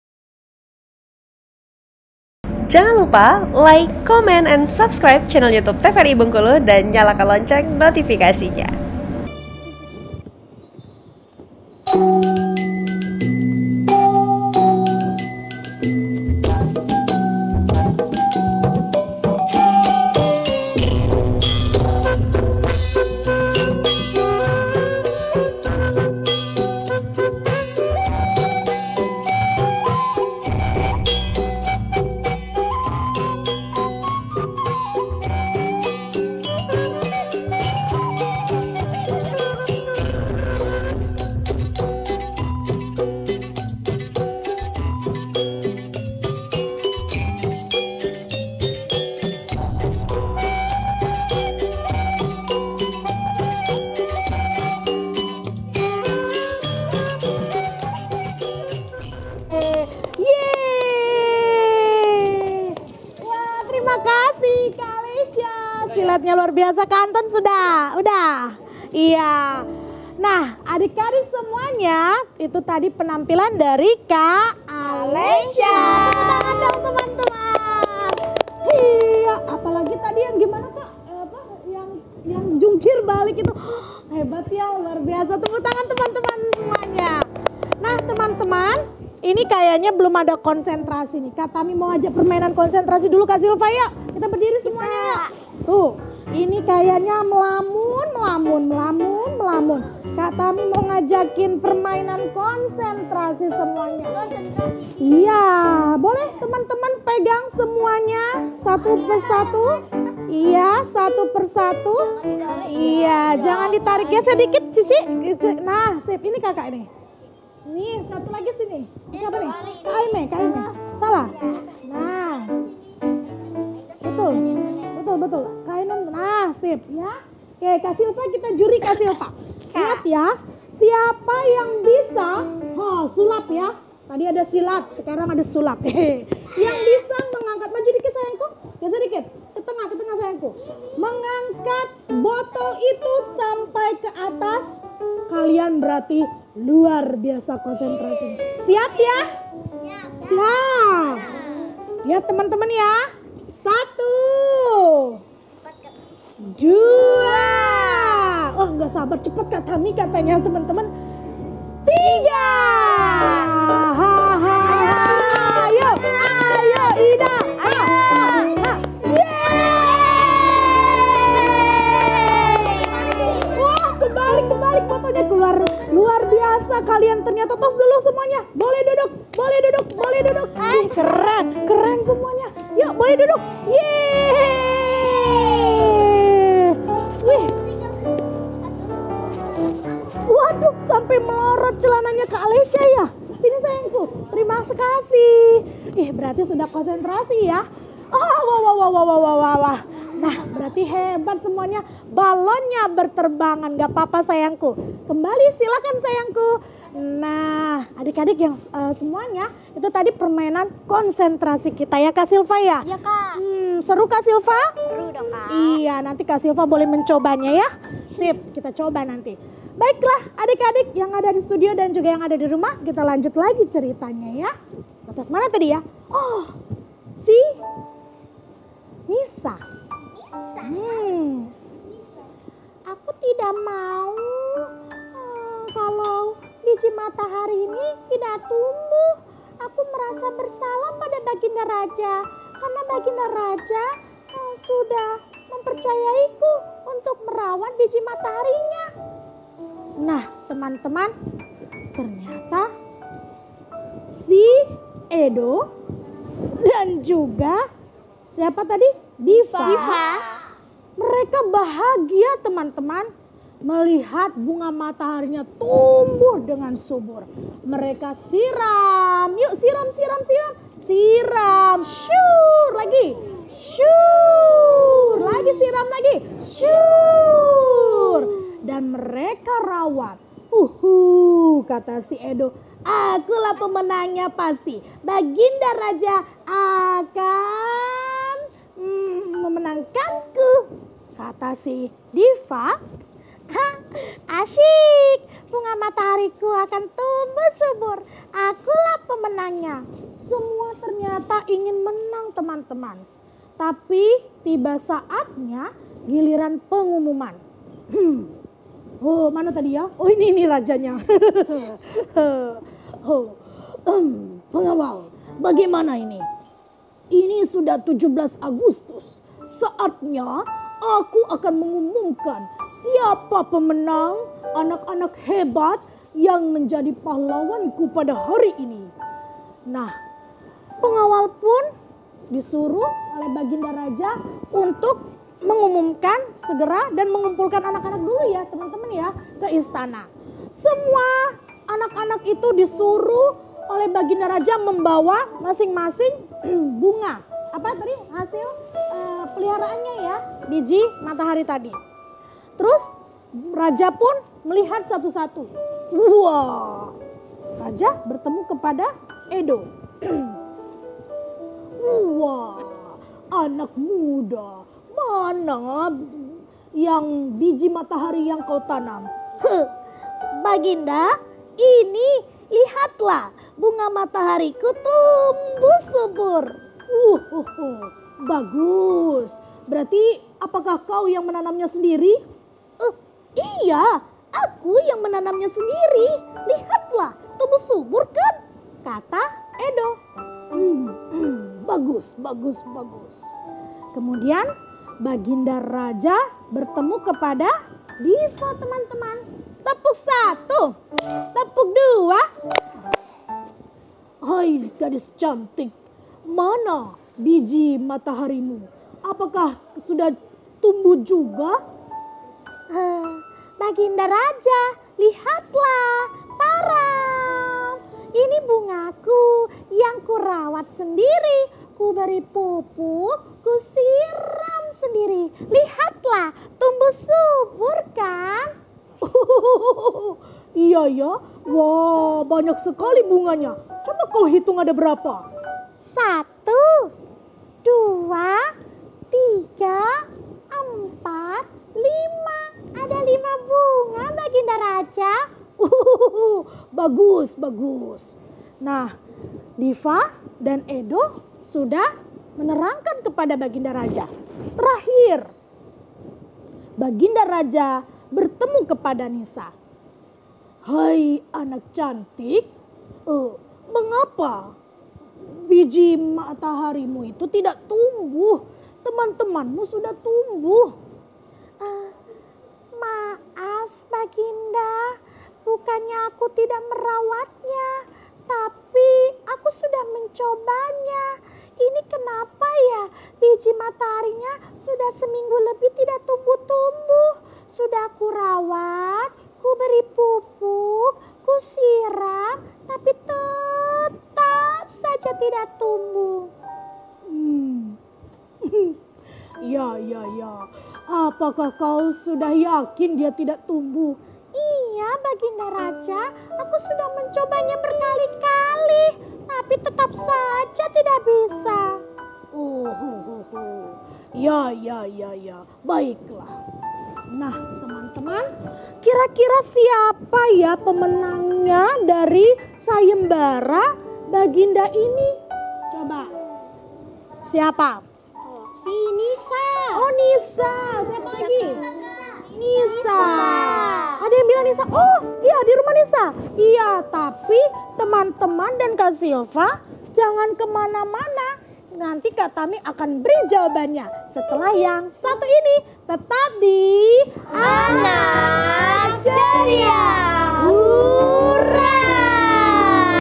DONGENG HARI KEMERDEKAAN SEGMEN 4
PENDONGENG:KAK TAMI
AUDIANCE  :SANGGAR BESAMO KITO